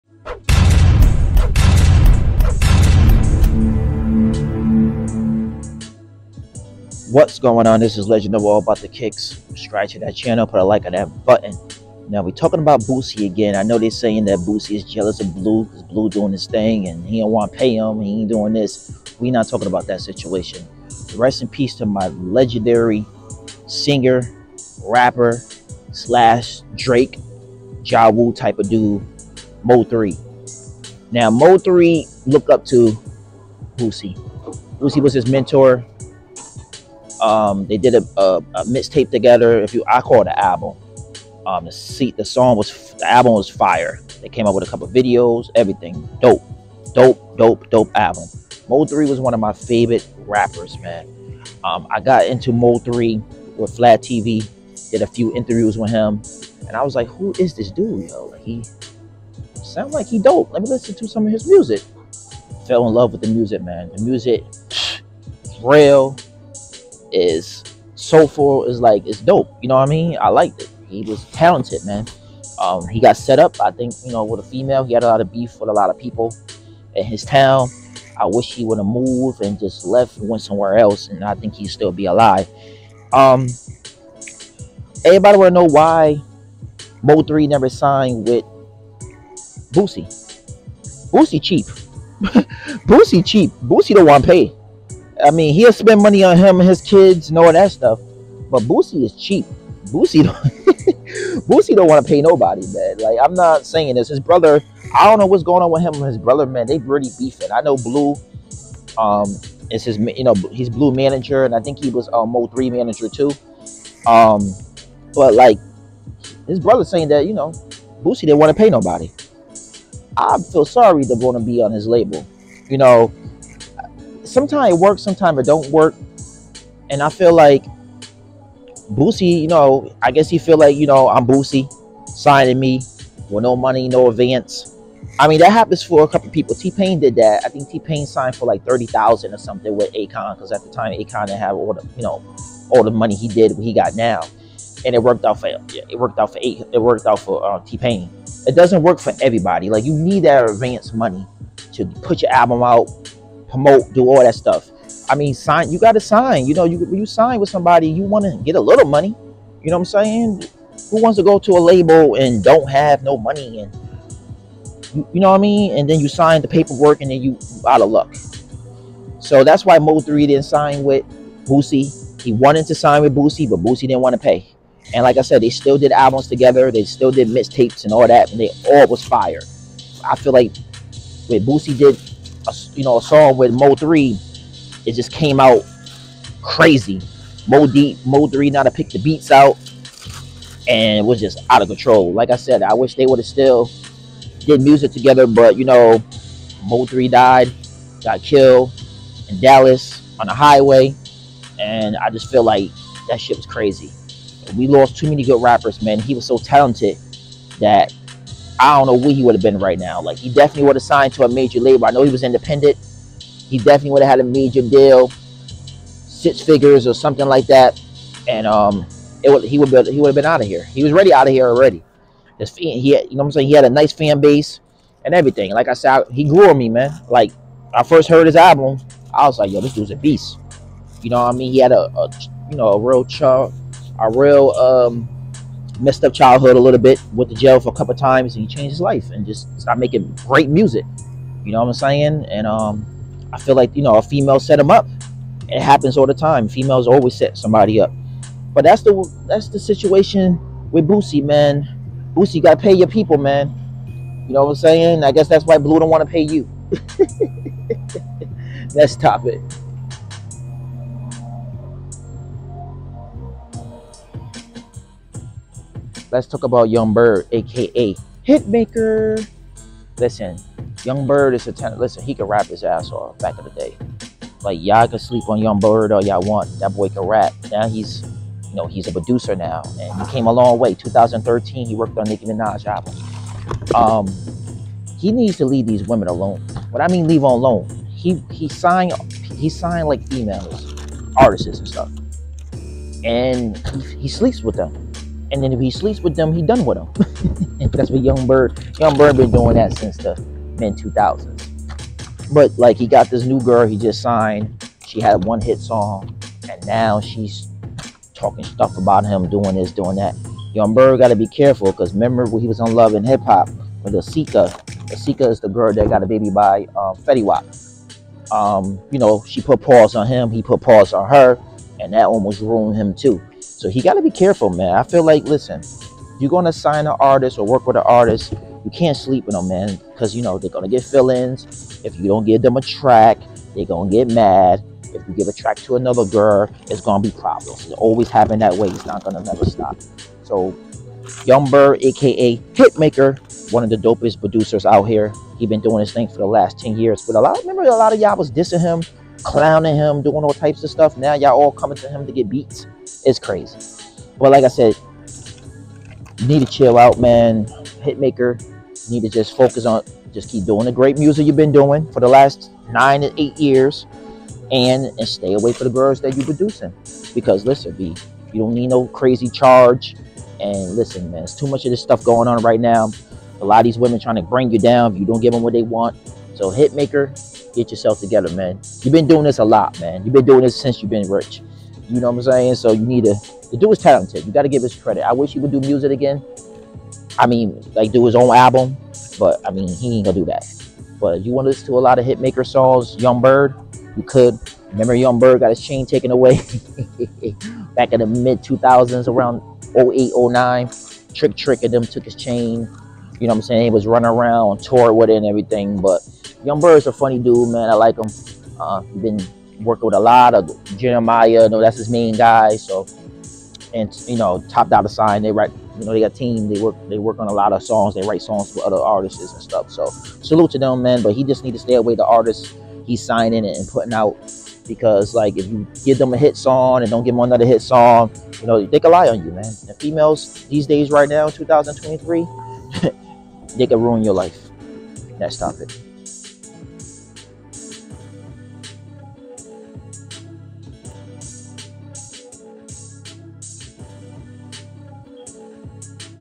Why didn’t MO3 sign with Boosie Tink Blasts hit￼maka aka YungBerg Over he Altercation Foul
Boosie is mad at Yung Bleu because he got a feature from Drake Why didn’t Mo three sign with Boosie￼￼